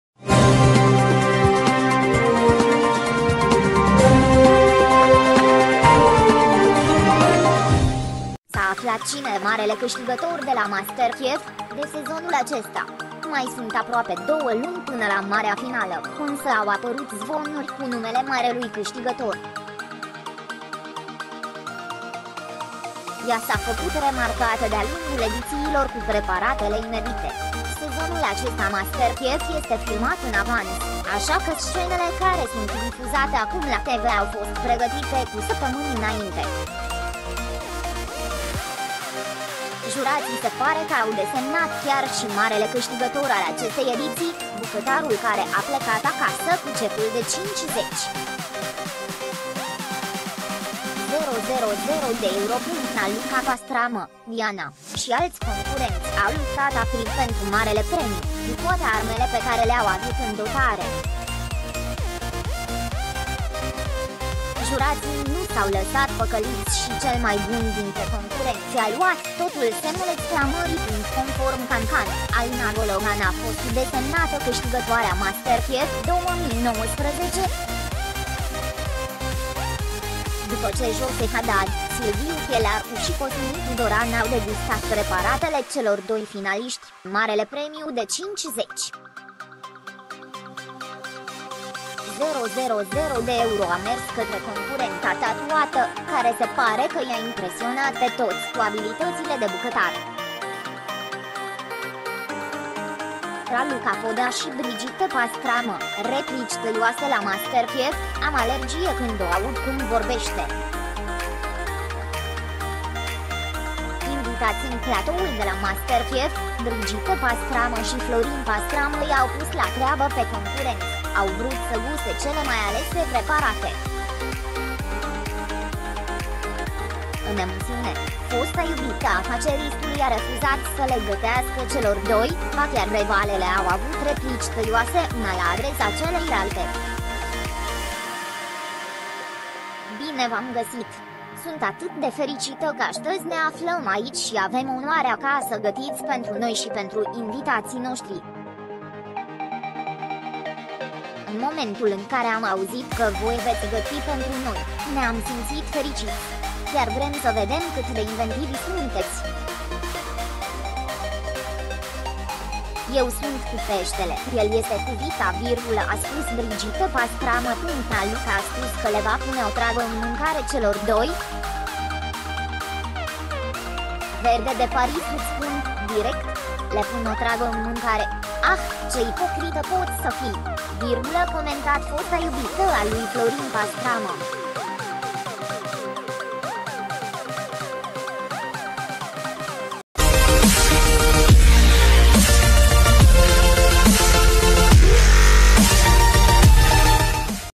Cine e marele câștigător de la Masterchef Ea a primit cecul în valoare de 50 000 de euro
S-a aflat cine e marele câștigător de la Masterchef, de sezonul acesta. Mai sunt aproape două luni până la marea finală, însă au apărut zvonuri cu numele marelui câștigător. Ea s-a făcut remarcată de-a lungul edițiilor cu preparatele inedite!
Sezonul acesta Masterchef este filmat în avans, așa că scenele care sunt difuzate acum la TV au fost pregătite cu săptămâni înainte. Jurații se pare că au desemnat chiar și marele câștigător al acestei ediții, bucătarul care a plecat acasă cu cecul de 50.000 de euro.

Raluca Pastramă, Diana, și alți concurenți au luptat aprig pentru marele premiu, cu toate armele pe care le-au avut în dotare. Jurații nu s-au lăsat păcăliți și cel mai bun dintre concurenți a luat totul!

Conform Cancan, Alina Gologan a fost desemnată câștigătoarea Masterchef 2019. După ce Joseph Hadad, Silviu Chelaru și Cosmin Tudoran au degustat preparatele celor doi finaliști, marele premiu de 50.000 de euro a mers către concurenta tatuată, care se pare că i-a impresionat pe toți cu abilitățile de bucătar.

Raluca Podea și Brigitte Pastramă, replici tăioase la Masterchef: „Am alergie când o aud cum vorbește”
Invitați în platoul de la Masterchef, Brigitte Pastramă și Florin Pastramă i-au pus la treabă pe concurenți, au vrut să guste cele mai alese preparate. În emisiune, fosta iubită a afaceristului a refuzat să le gătească celor doi, ba chiar revalele au avut replici tăioase una la adresa celeilalte.
„Bine v-am găsit! Sunt atât de fericită că astăzi ne aflăm aici și avem onoarea ca să gătiți pentru noi și pentru invitații noștri. În momentul în care am auzit că voi veți găti pentru noi, ne-am simțit fericiți. Chiar vrem să vedem cât de inventivi sunteți. Eu sunt cu peștele, el este cu vita”, a spus Brigitte Pastramă.

Raluca a spus că le va pune otravă în mâncare celor doi. „Verde de Paris îți pun, direct. Le pun otravă în mâncare. Ah, ce ipocrită poți să fii!”, comentat fosta iubită a lui Florin Pastramă.